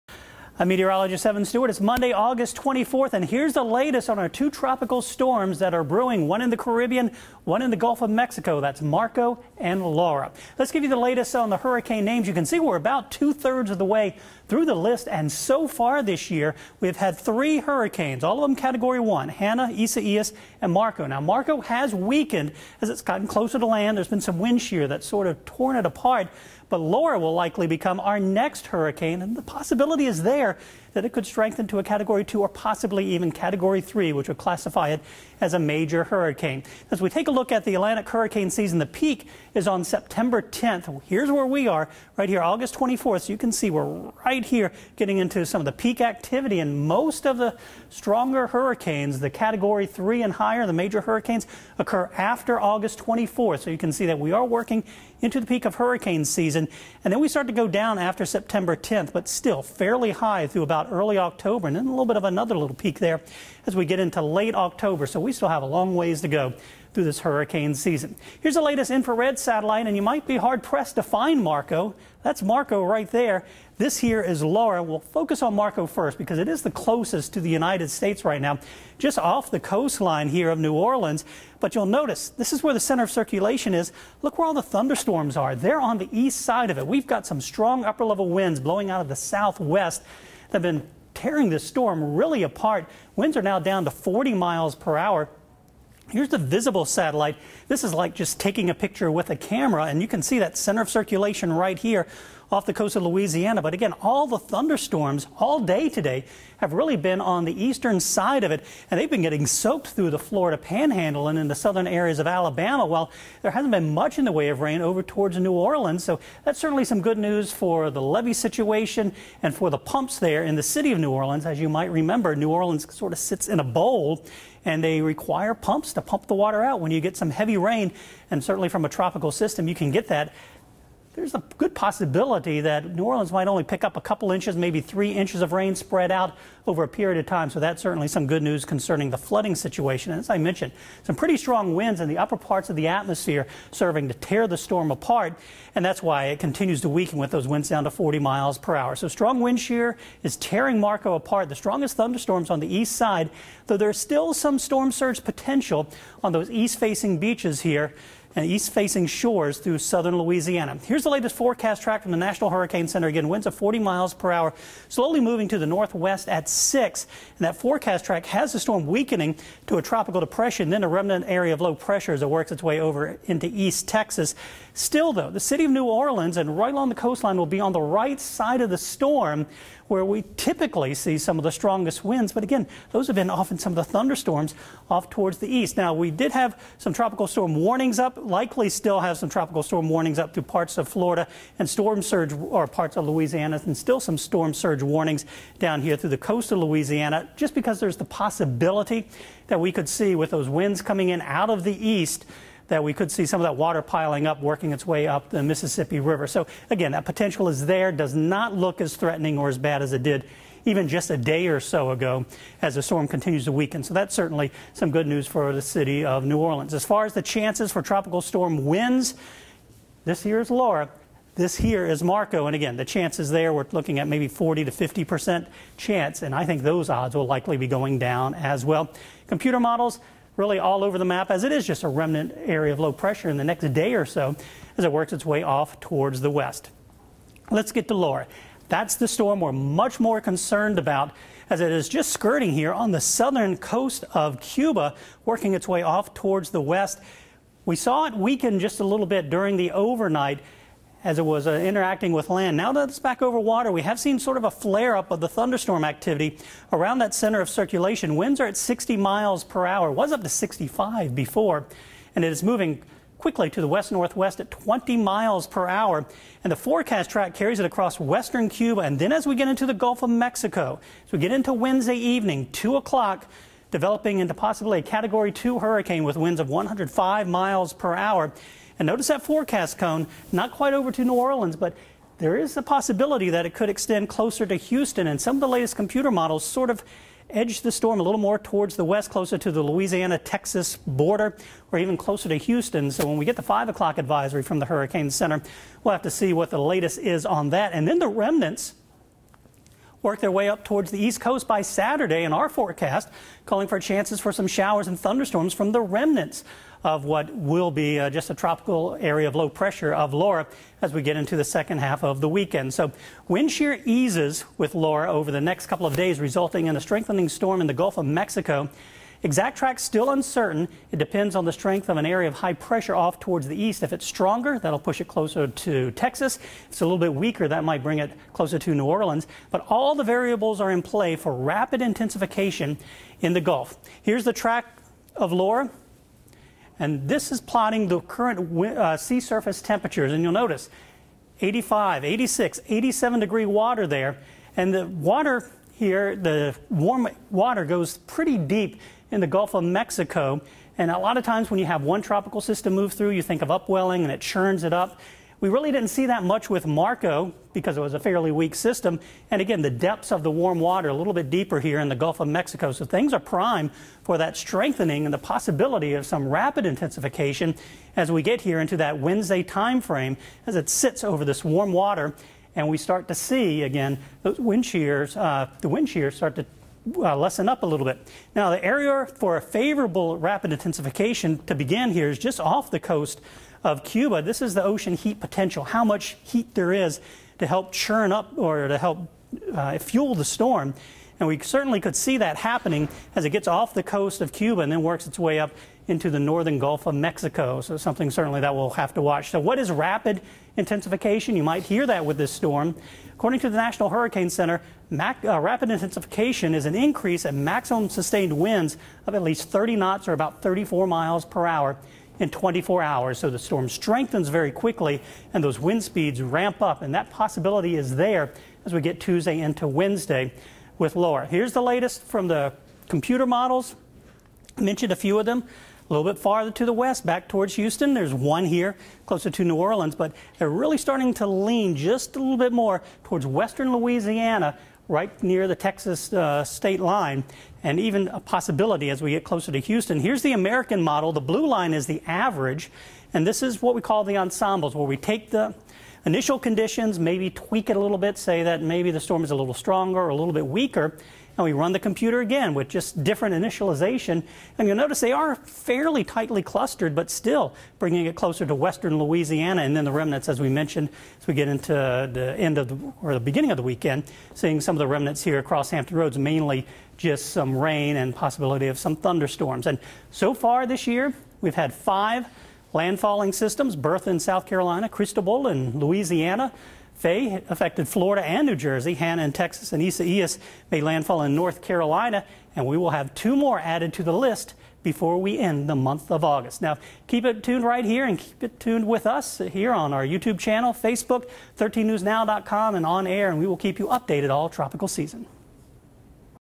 Tropics Update: Tropical Storm Marco nears landfall, Tropical Storm Laura not far behind
13News Now Meteorologist Evan Stewart gives a tropics update for Monday, August 24, 2020, on Tropical Storm Marco and Tropical Storm Laura, which are both taking aim at the Gulf Coast.